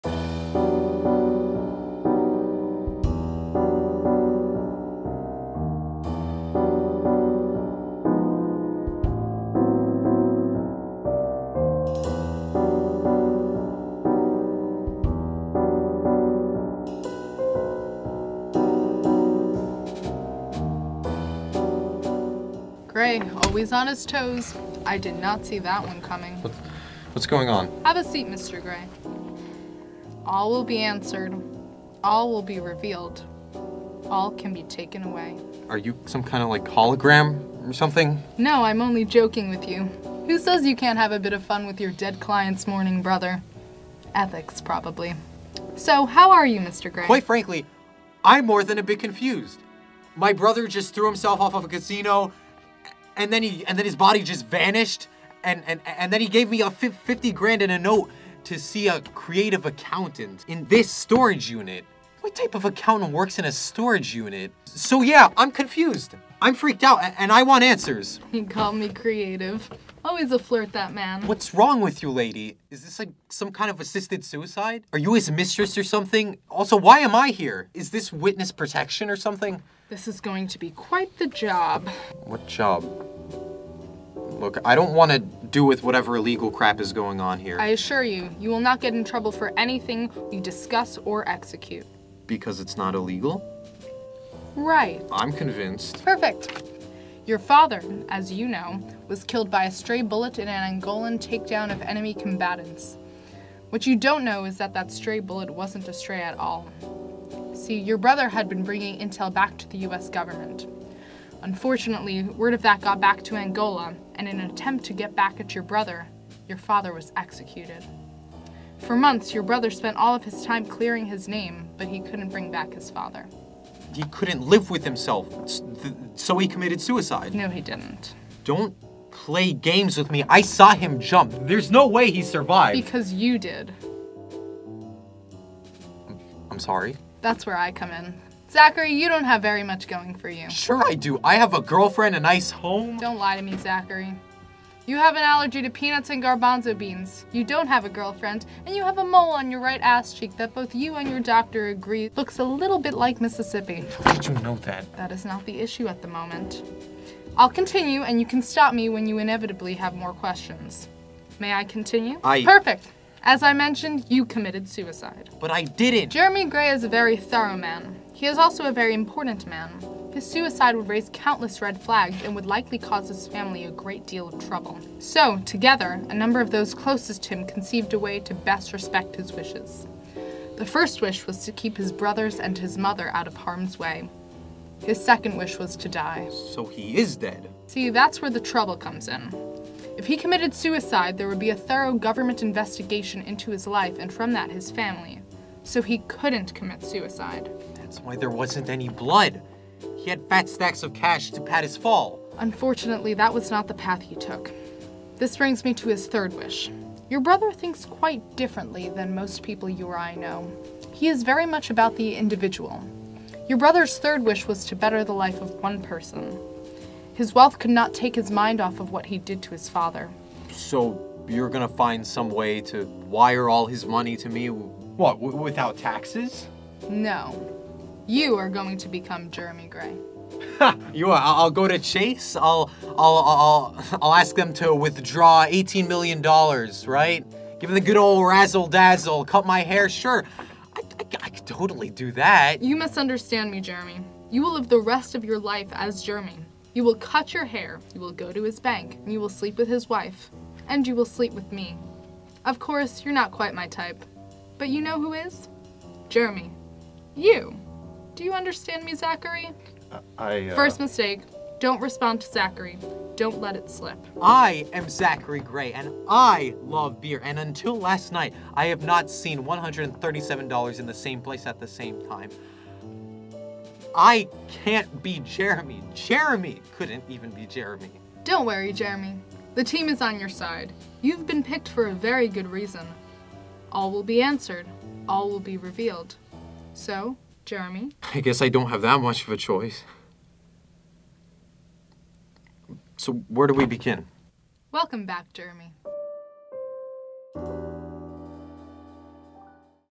To Become Jeremy Gray (a short film)
To Become Jeremy Gray is a scene from a feature film that I am in the process of writing. I felt that filming a scene from it would give me a push to keep writing.

Writer: Adam J Sperling
Director: Adam J Sperling
Score: Devin Chin

Starring:
Victor Polizu as Zachary Gray
Talia Katz as A Creative Accountant

Cinematography, Audio, and Editing by Adam J Sperling